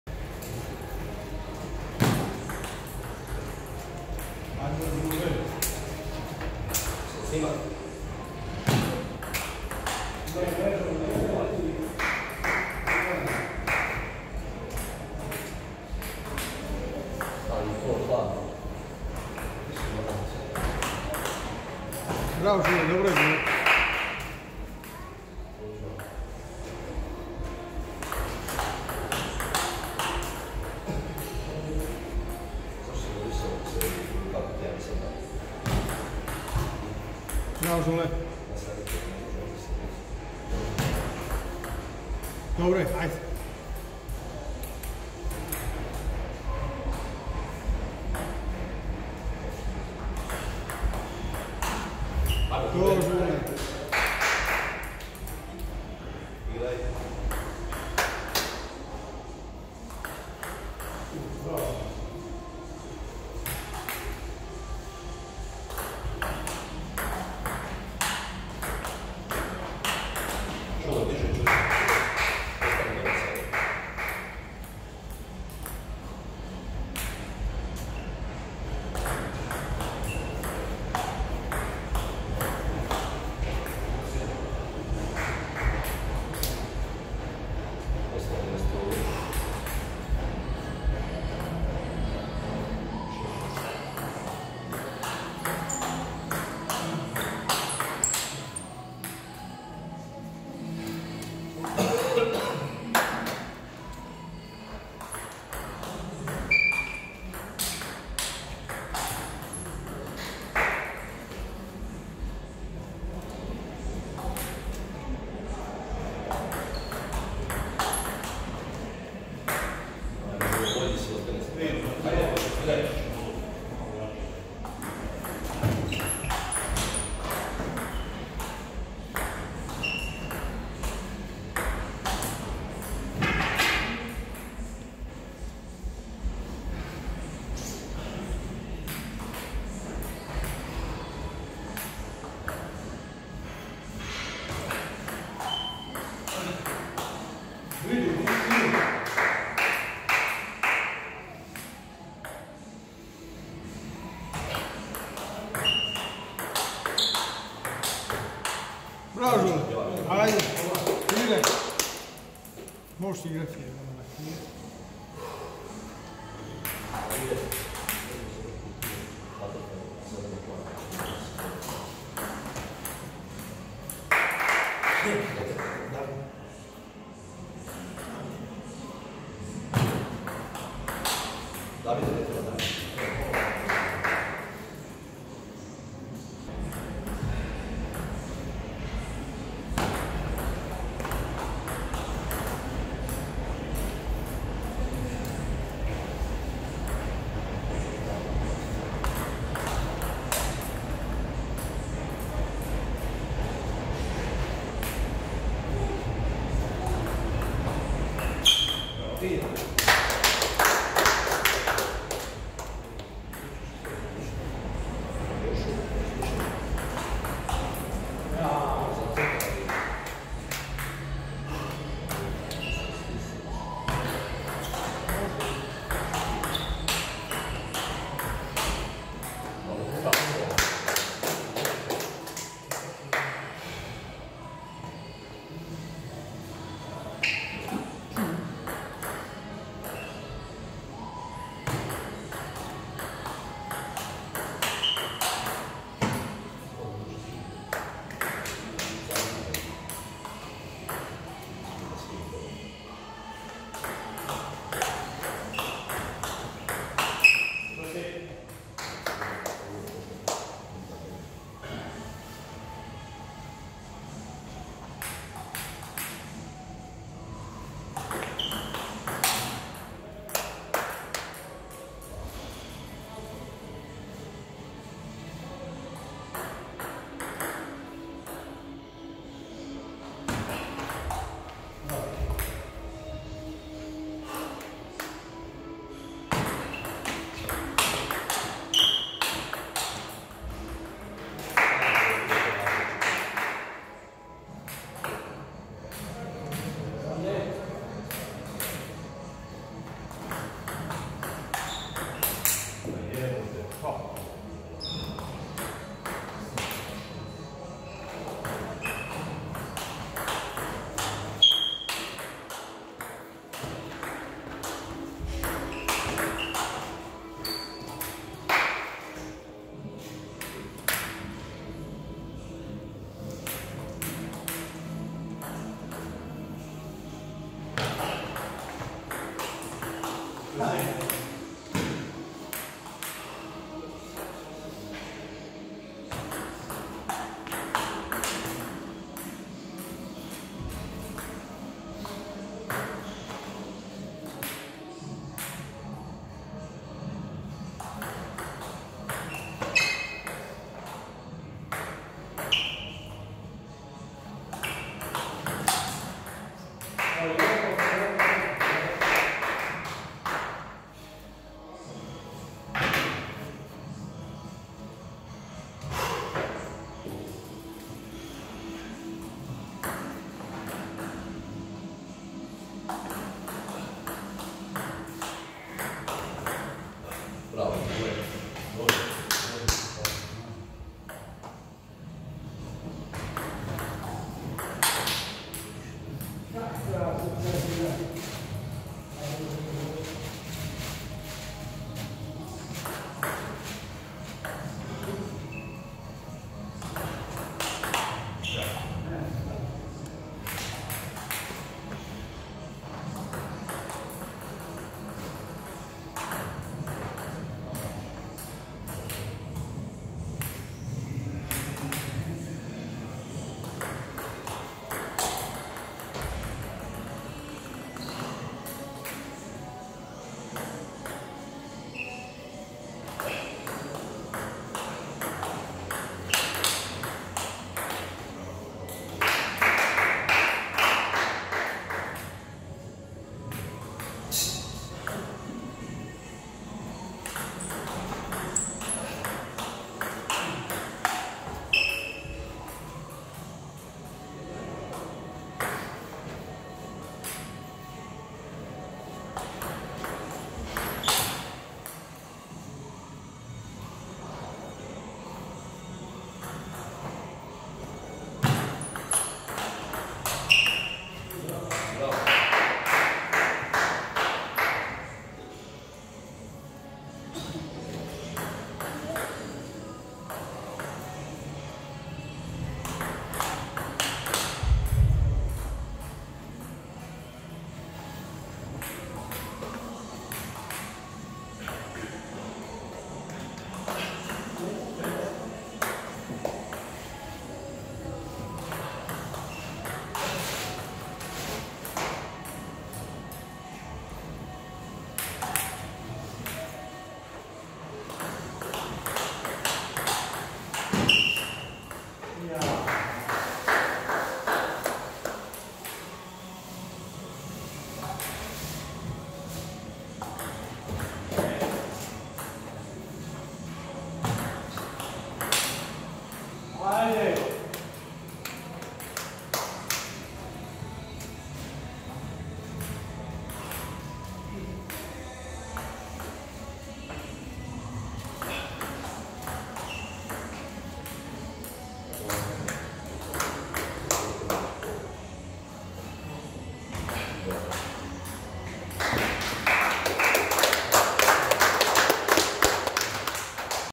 ŽUPANIĆ Željko- JURINEC Rinaldo 0:3, 10.kolo, 1.HL liga istok, 23.03.2019.
PO SETOVIMA - 7:11 , 5:11 , 5:11
UTAKMICA - STK Cibalia Vinkovci / STK PUTJANE Čakovec
REZULTAT - 2:4
GRAD DOMACIN - Vinkovci
DVORANA - Fitness centar Zara
SUDAC - Dragicevic M.